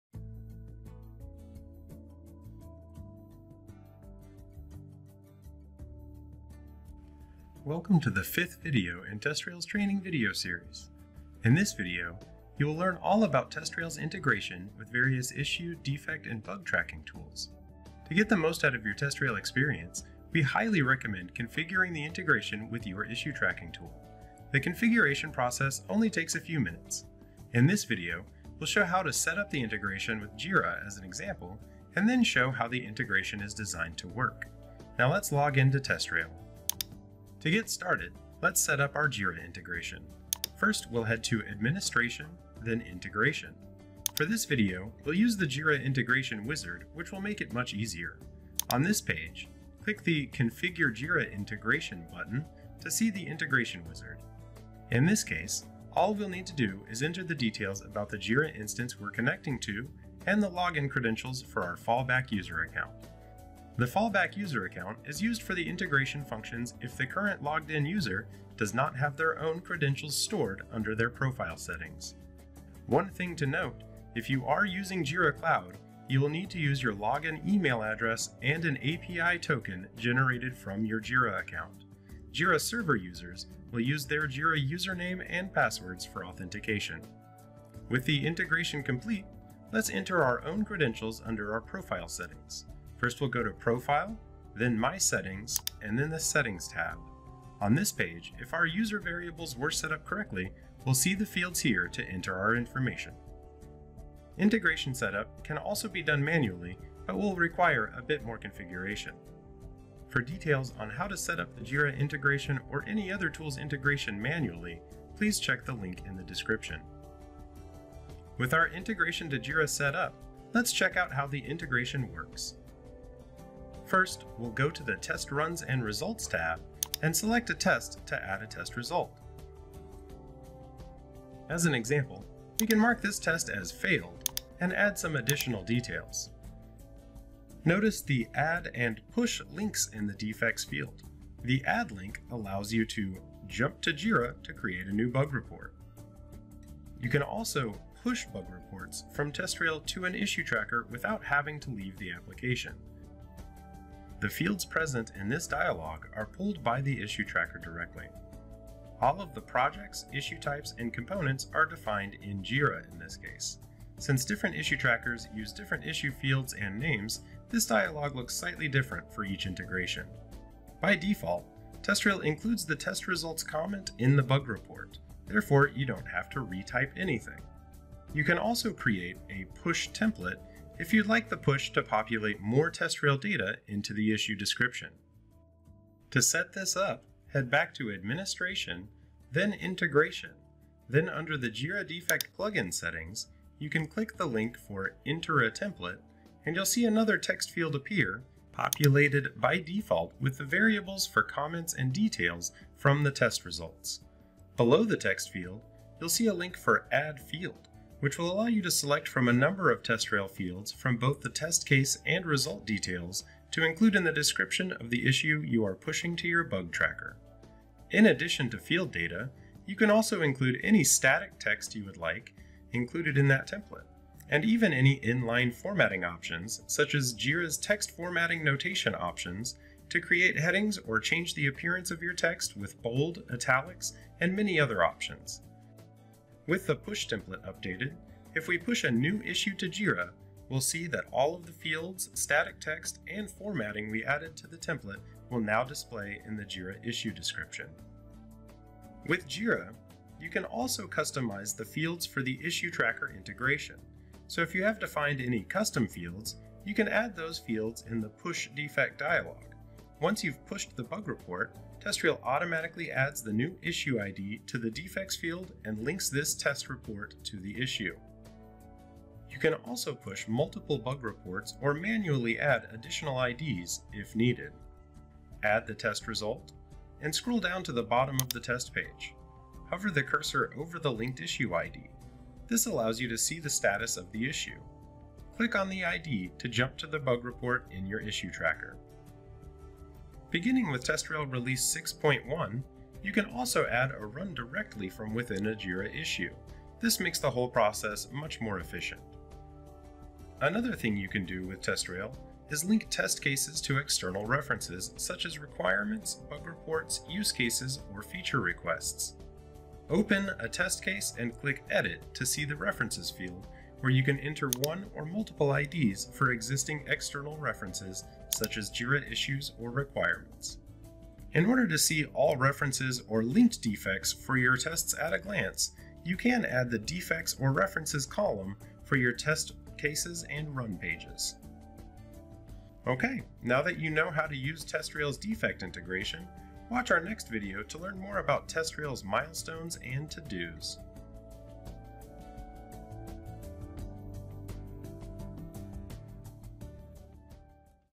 TestRail's Defect Integration
One of the things that make TestRail so popular is its seamless integration with issue, defect, and bug tracking tools, such as Jira, Youtrack, GitHub and many more. In this video, you will learn all about TestRail’s defect integration.

⚡Introduction to reference and defect integrations
⚡Webinar: Full Traceability for All with the Jira Integration